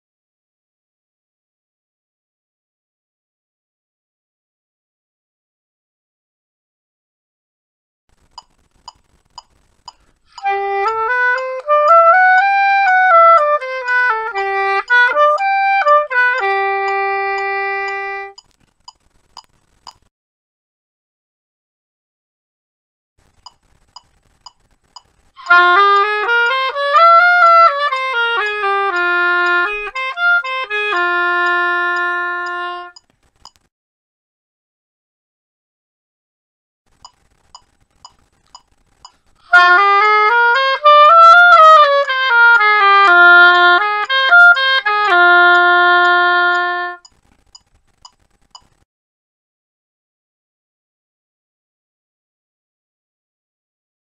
Playing Assignment #1 (Scales) [Wind Ensemble]
This video was created for the sole purpose of completing a playing/recording assignment for the American Canyon High School Chamber String Orchestra as a student in the ACHS Chamber Orchestra, under the direction of music director Matthew Smith.